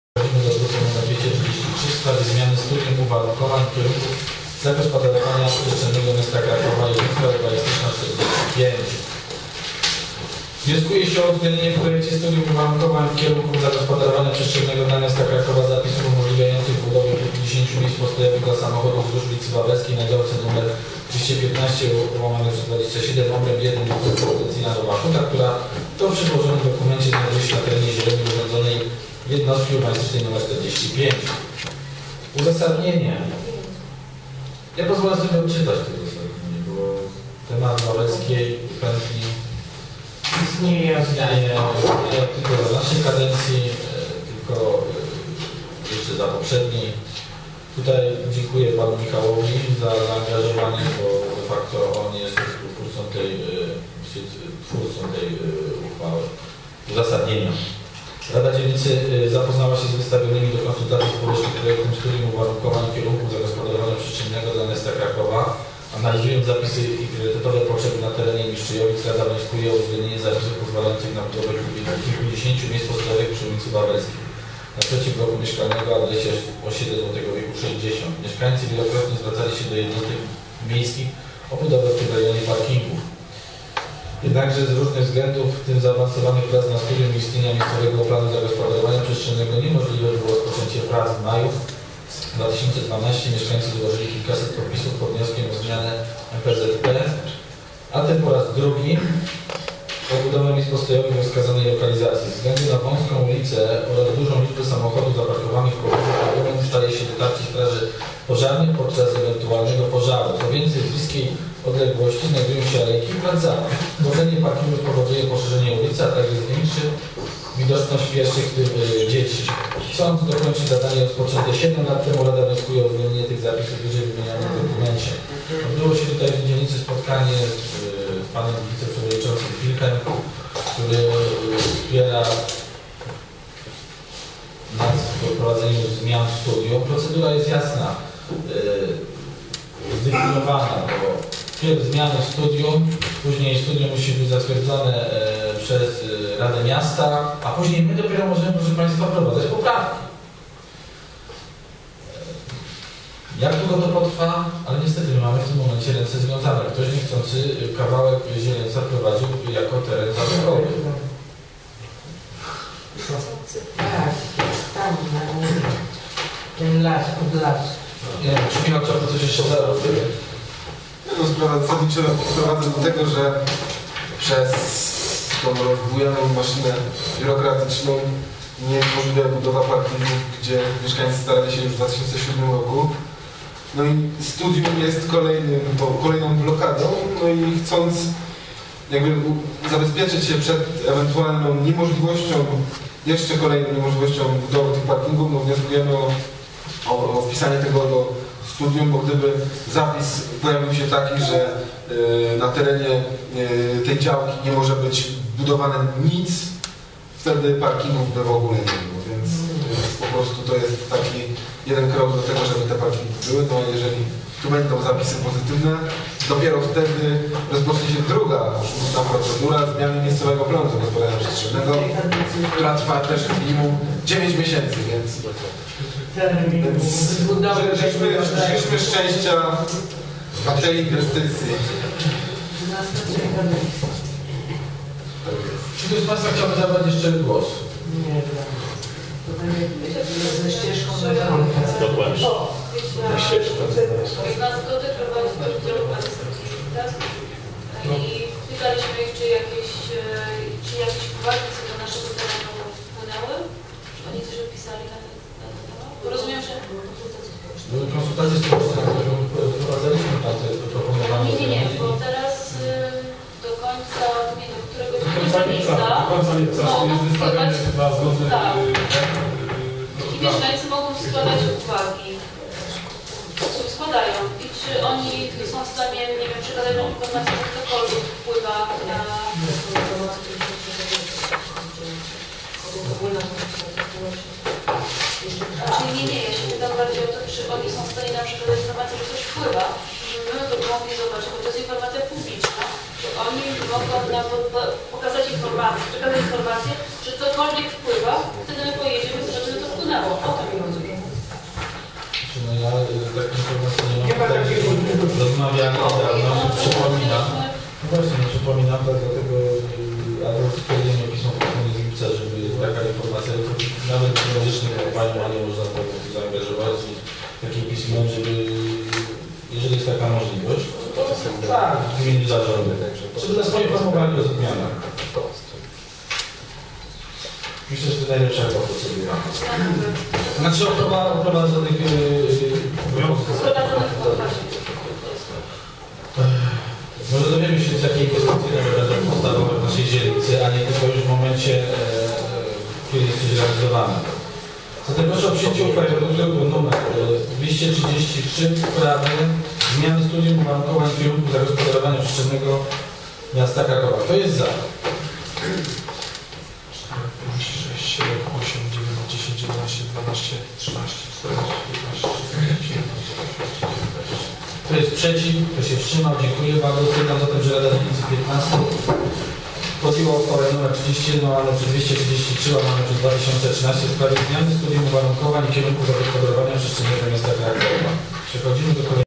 RD_XV_11-07-2013_11 Uchwała ws. zmiany Studium - jednostka urbanistyczna 45
Obrady XXXI sesji Rady Dzielnicy XV Mistrzejowice z dn. 11 lipca 2013 r.